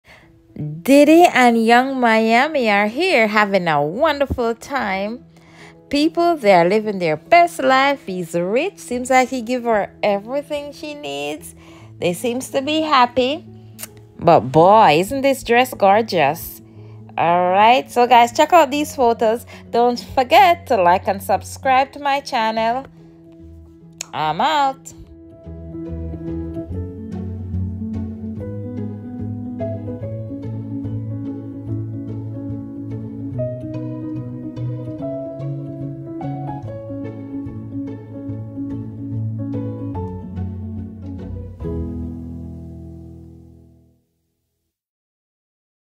Diddy & Yung Miami Having a great time in St Barts on his Yacht on New Year’s Eve
Diddy & Young Miami seems to be having a wonderful time, on New Year’s Eve.